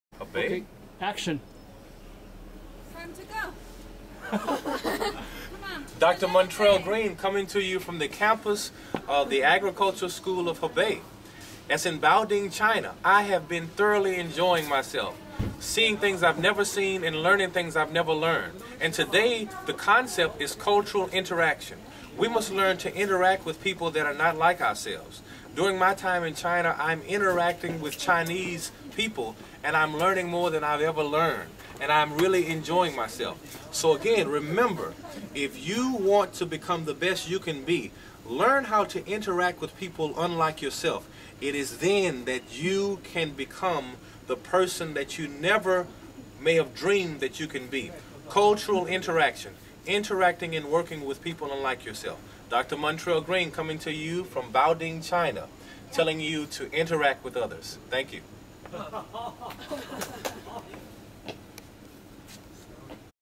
Cultural Interaction
Food for thought produced in Boading China in December 2009.  When we work with people unlike ourselves (thought, looks, background), we bring out the best in ourselves.  Teamwork makes the Dreamwork.